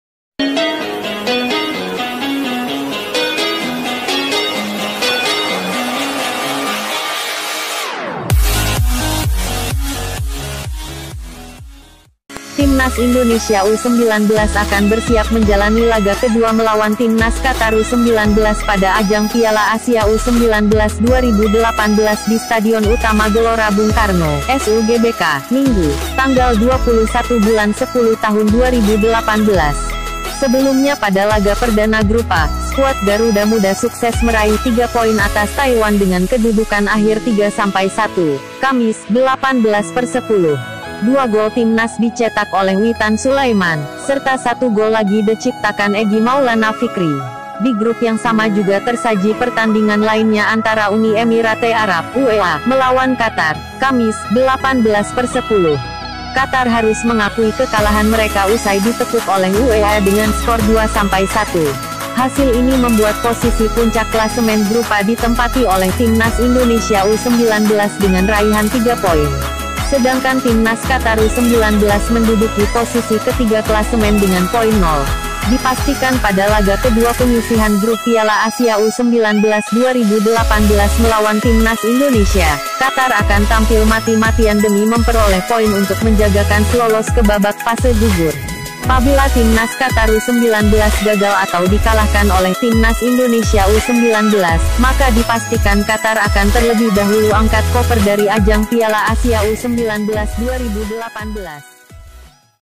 Lawan Timnas Indonesia U-19, Qatar Dipastikan Tampil Habis-Habisan Agar Tak Langsung Angkat Koper
Timnas Indonesia U-19 akan bersiap menjalani laga kedua melawan timnas Qatar U-19 pada ajang Piala Asia U-19 2018 di Stadion Utama Gelora Bung Karno (SUGBK), Minggu (21/10/2018).

Sebelumnya pada laga perdana grup A, skuat Garuda Muda sukses meraih tiga poin atas Taiwan dengan kedudukan akhir 3-1, Kamis (18/10).

Dua gol timnas dicetak oleh Witan Sulaiman, serta satu gol lagi dciptakan Egy Maulana Vikri.

Baca Juga :

Pelatih Lechia Gdansk Punya Pesan Untuk Egy Maulana di Piala Asia U-19

Pelatih Taiwan Mengaku Berikan Instruksi Khusus Untuk Egy Maulana Vikri

Lechia Gdansk Soroti Kesuksesan Egy Maulana Bawa Timnas U-19 Menang atas Taiwan

Di grup yang sama juga tersaji pertandingan lainnya antara Uni Emirate Arab (UEA) melawan Qatar, Kamis (18/10).

Qatar harus mengakui kekalahan mereka usai ditekuk oleh UEA dengan skor 2-1.

Hasil ini membuat posisi puncak klasemen grup A ditempati oleh Timnas Indonesia U-19 dengan raihan tiga poin.

Sedangkan timnas Qatar U-19 menduduki posisi ketiga klasemen dengan poin 0.

Dipastikan pada laga kedua penyisihan grup Piala Asia U-19 2018 melawan timnas Indonesia, Qatar akan tampil mati-matian demi memperoleh poin untuk menjaga kans lolos ke babak fase gugur.

Apabila timnas Qatar U-19 gagal atau dikalahkan oleh timnas Indonesia U-19, maka dipastikan Qatar akan terlebih dahulu angkat koper dari ajang Piala Asia U-19 2018.